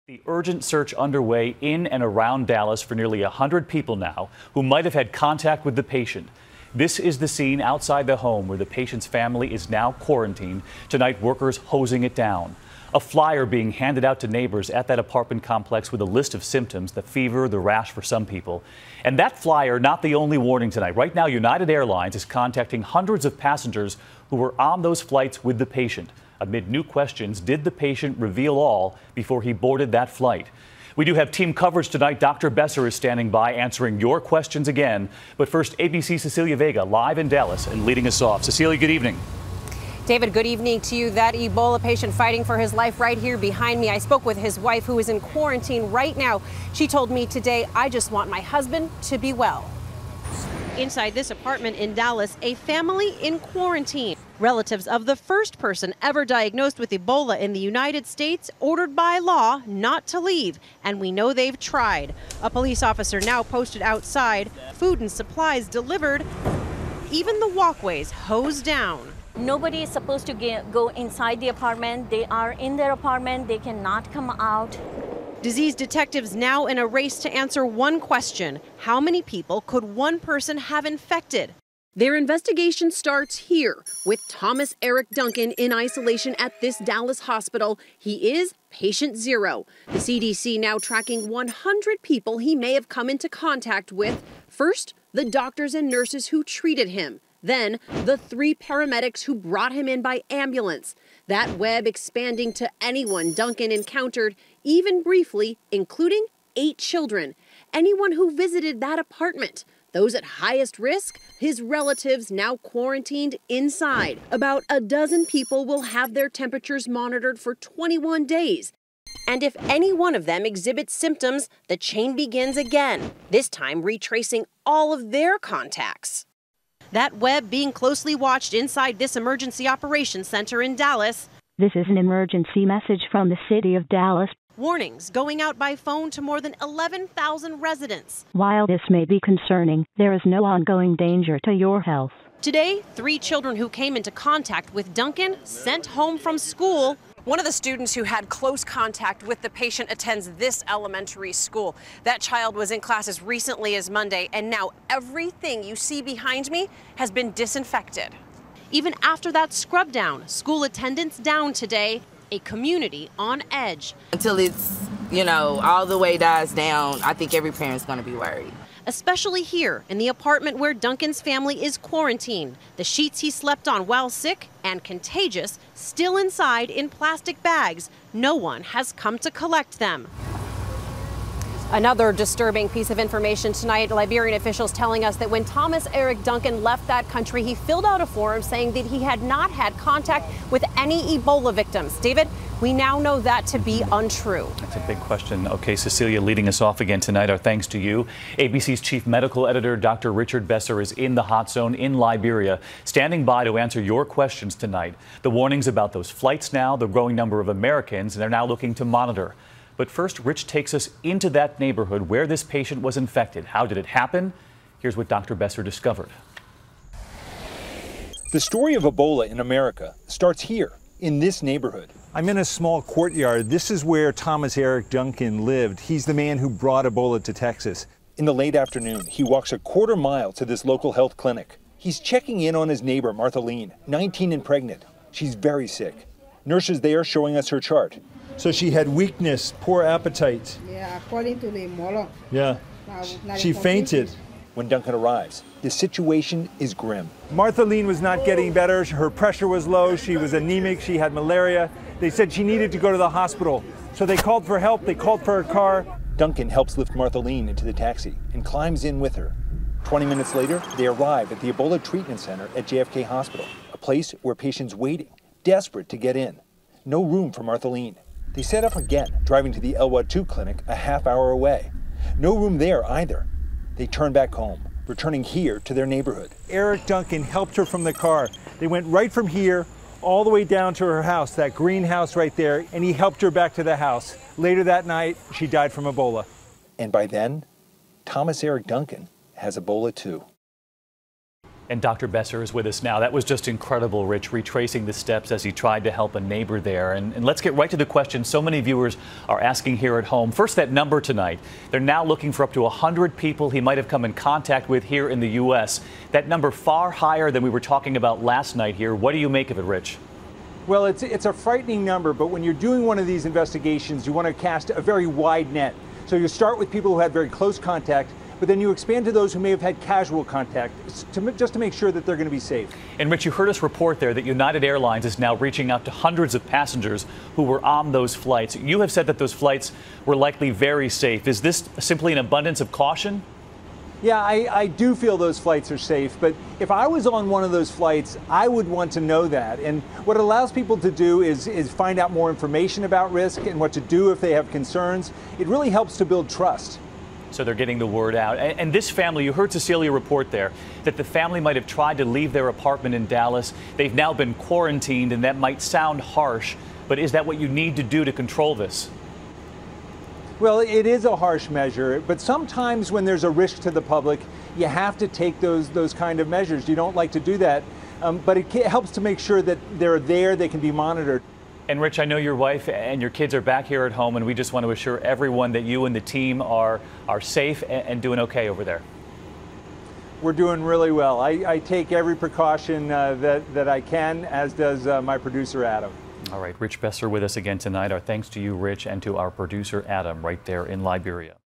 Stopping the Spread of the Deadly Virus From the Ebola Hot Zone to the United States
Urgent search for 100 people who may have come in contact with Ebola patient in the United States.